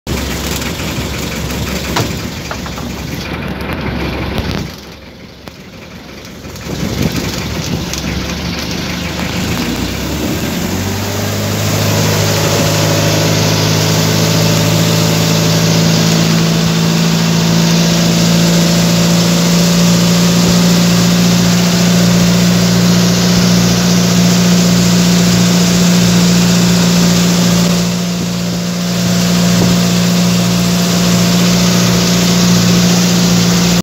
Puli Ta! From Bayas Island to Estancia Port
Uwian mode na.
After fieldwork, Packed up ang go home.

Route: Bayas island To Estancia Iloilo proper port.

Travel Vlog!!